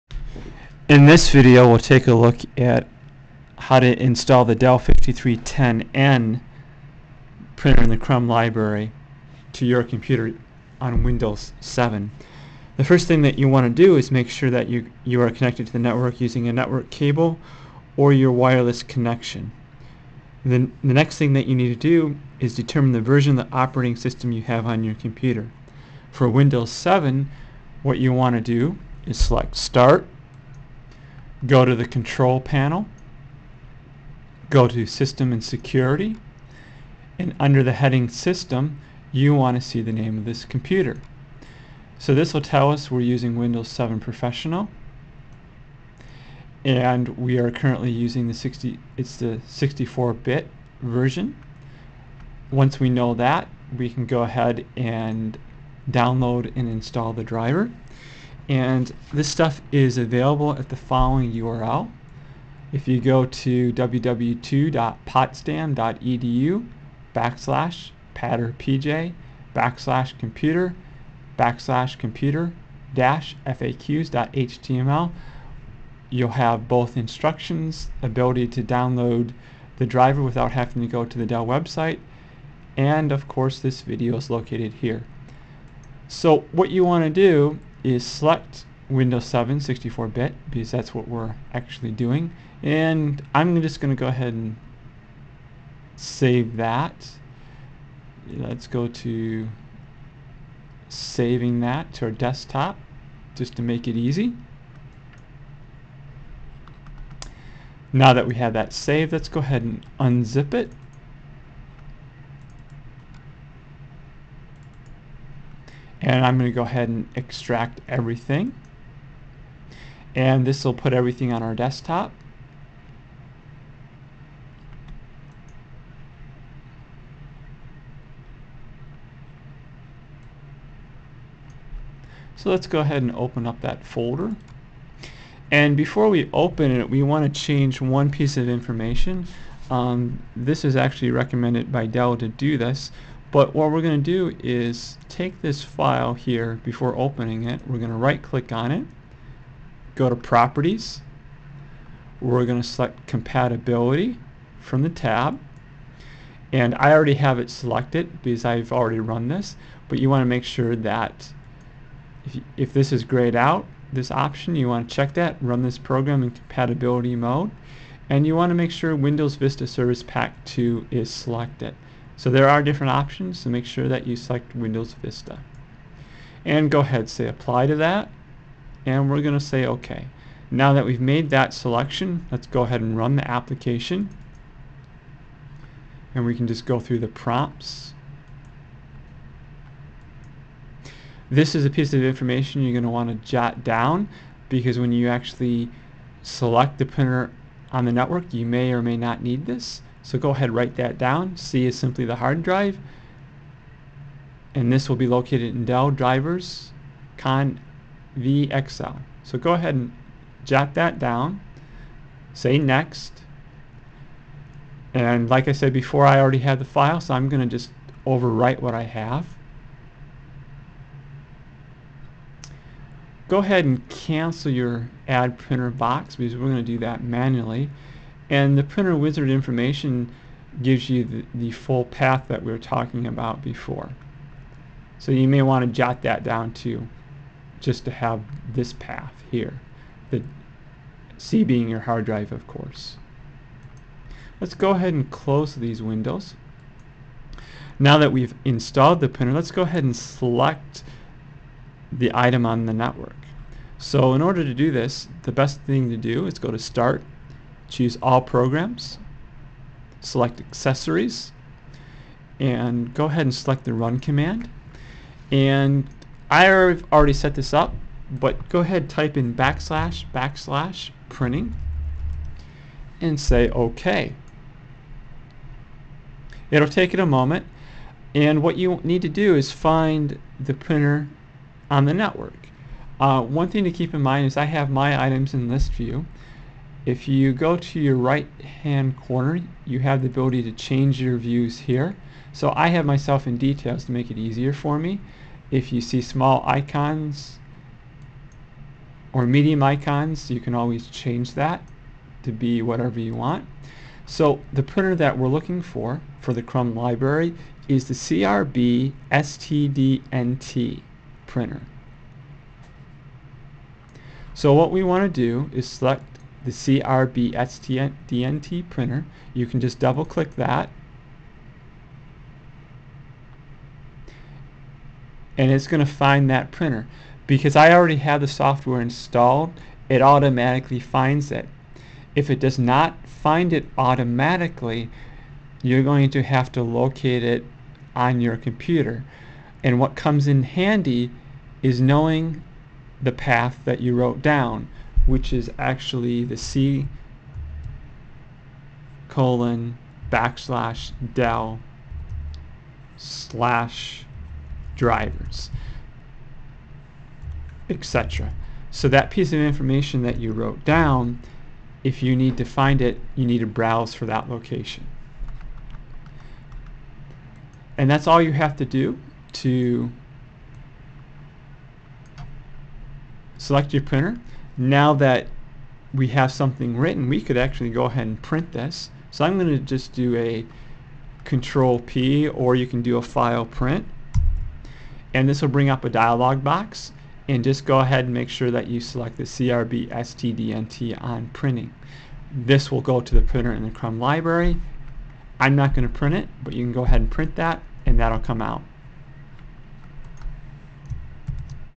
Add Dell 5310n Printer to PC
This video will show you how to add the Dell 5310n network printer located in the Crumb Library to a PC running the Windows 7 64 bit operating system.